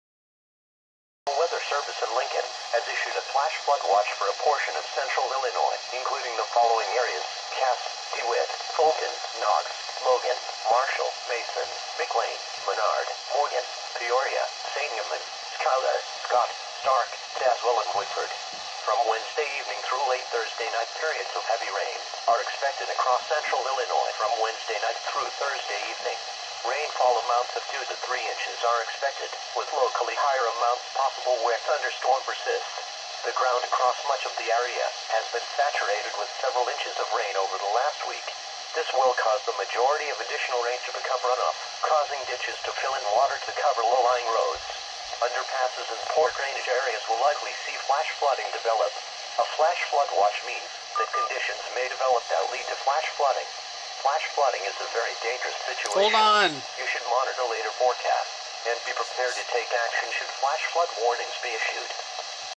Flash Flood Watch (NO EAS) 4/16/13
Issued before the extreme flooding event of April 17-18,2013 in Illinois. Issued by the NWS in Lincoln,IL.